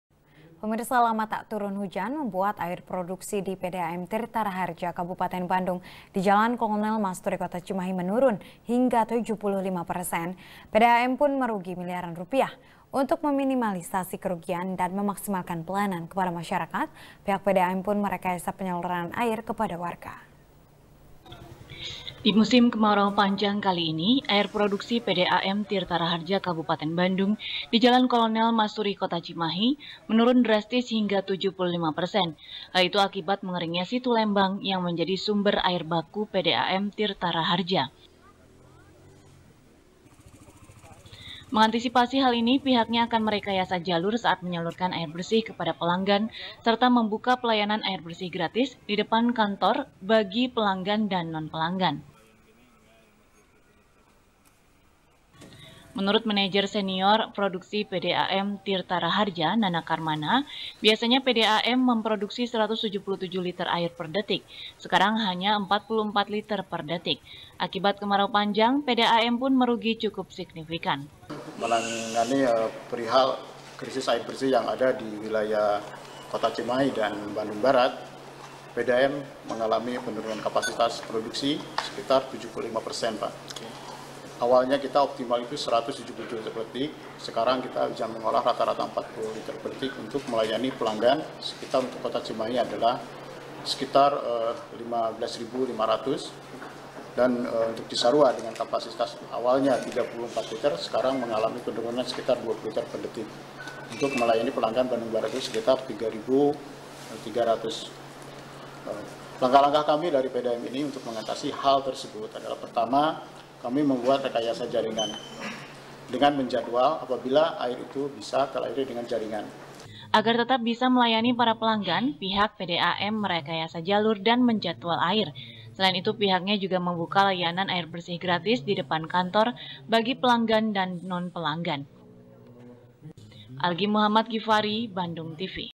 Kemarau Panjang, Produksi Air PDAM Menurun 75 Persen | SBR | BANDUNGTV
SBR | BANDUNGTV | NIKEN PRATIWI | Lama tak turun hujan, membuat air produksi di pdam tirta raharja, kabupaten bandung, di jalan kolonel masturi kota cimahi, menurun hingga 75 persen. pdam pun merugi miliaran rupiah. untuk meminimalisi kerugian dan memaksimalkan pelayanan kepada masyarakat, pihak pdam pun merekayasa penyaluran air kepada warga.